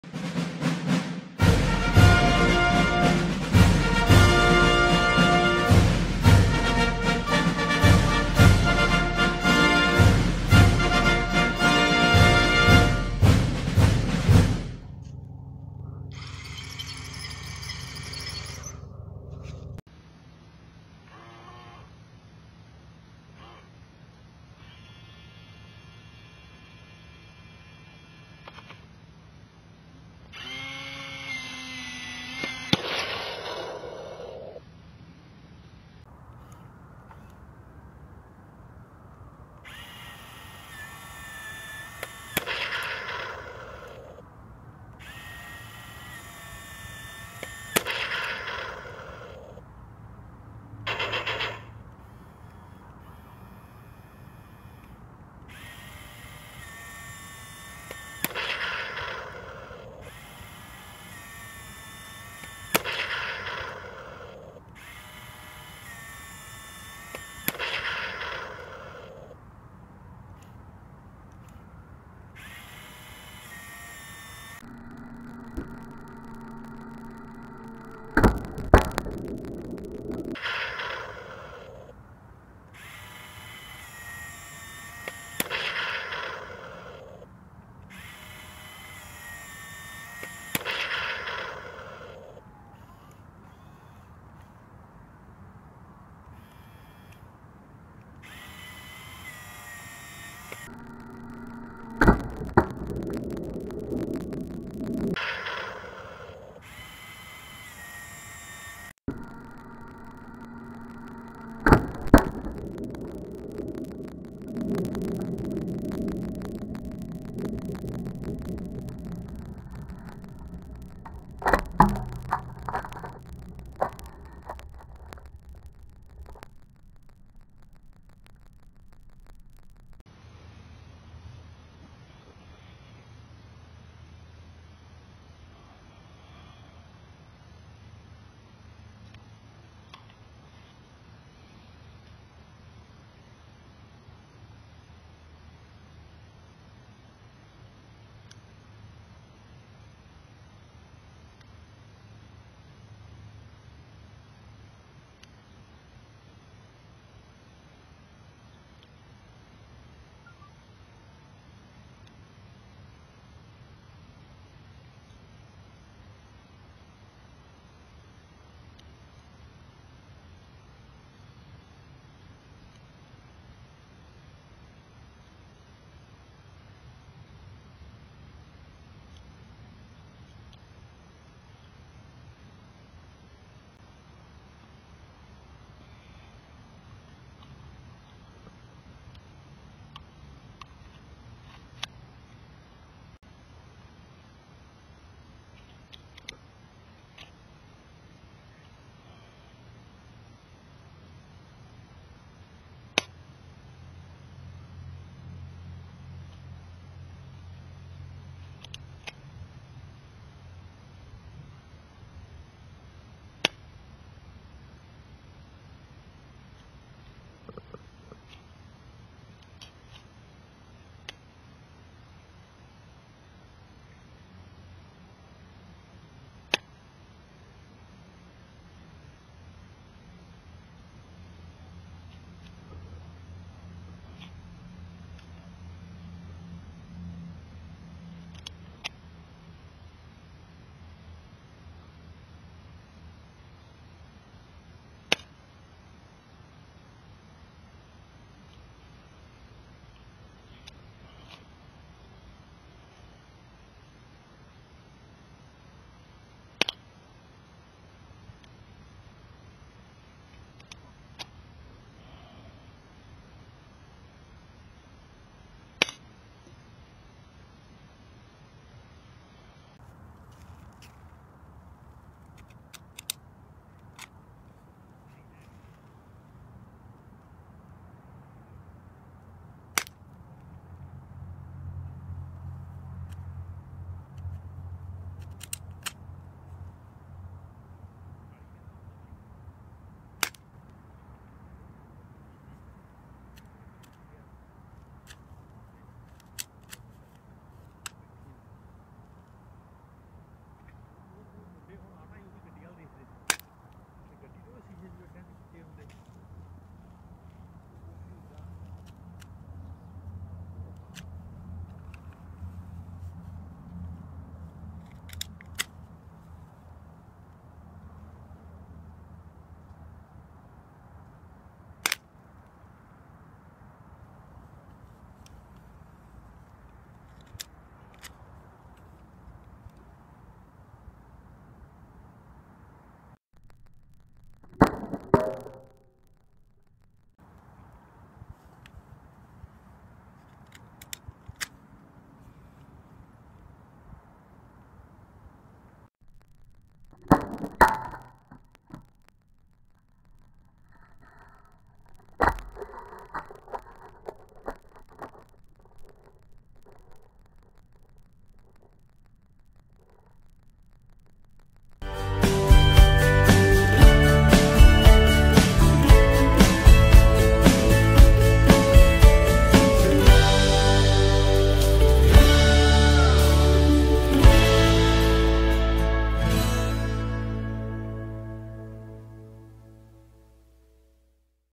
Air Pistol VS RC Tank
neither could gain the upper hand.